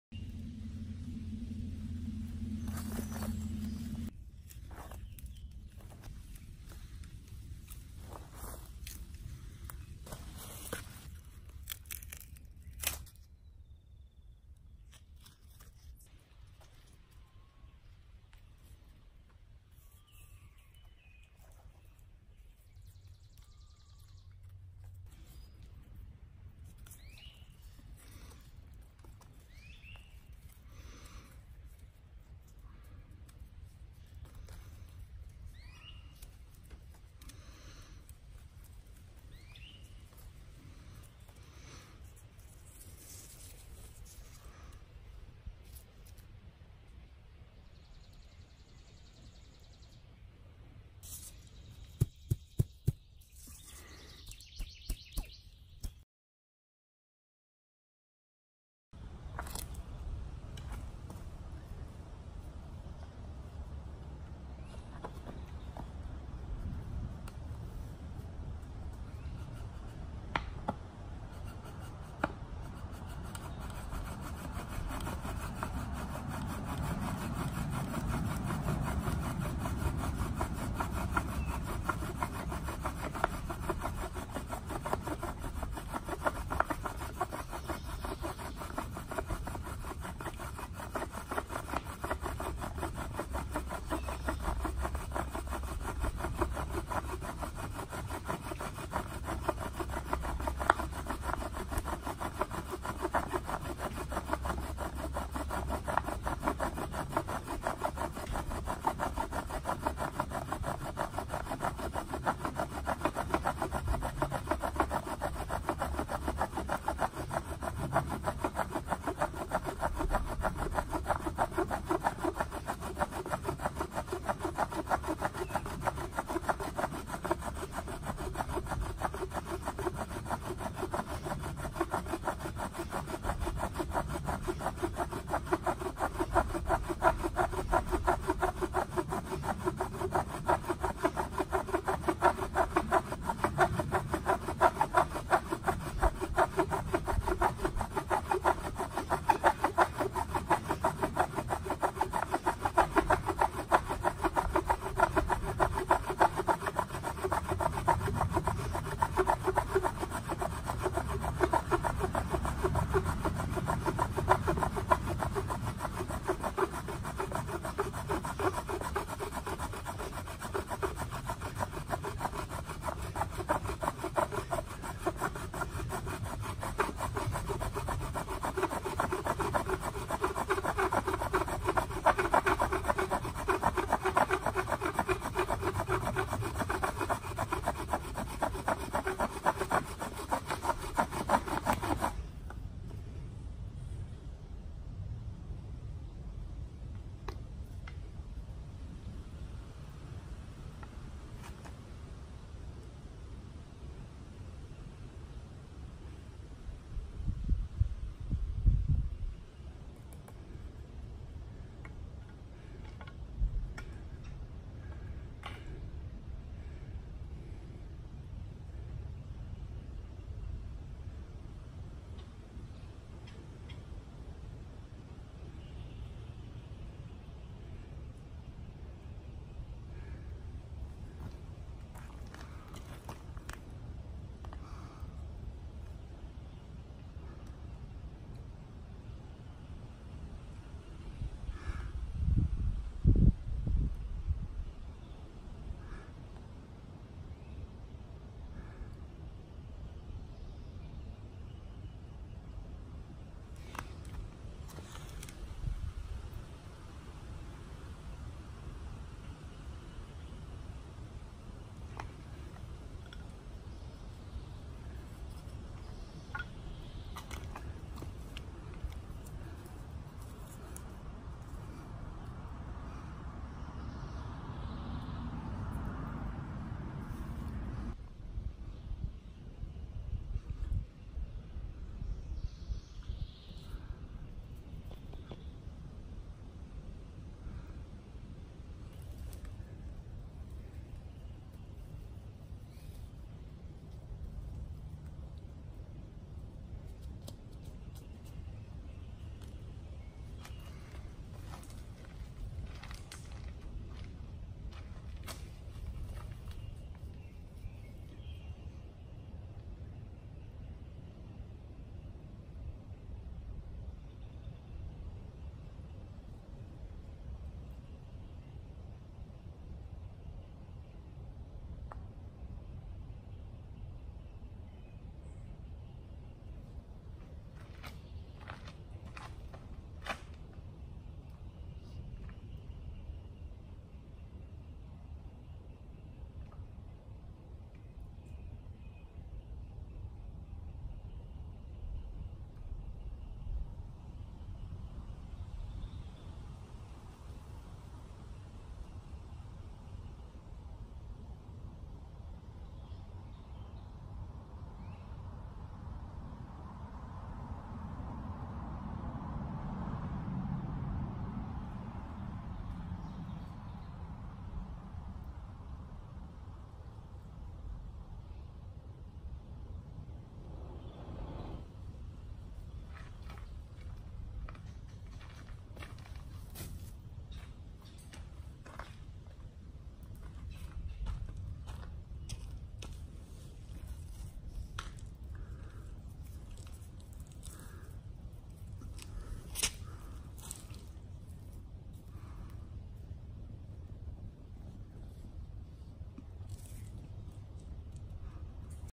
Braiding cottonwood tree bark cordage for bowdrill friction fire
Braiding tree bark cordage for a bowdrill friction fire. Cottonwood cordage with box elder on cedar.